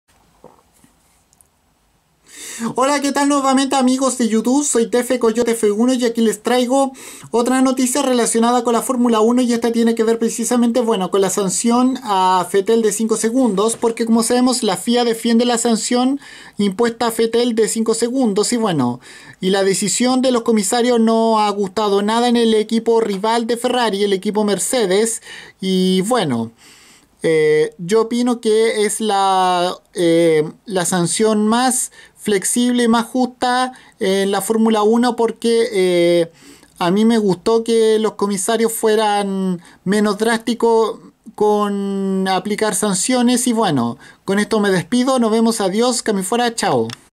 La Fia defiende la sanción de 5 segundos impuesta a Vettel
En la escuderia Mercedes esta decisión no le ha gustado a nadie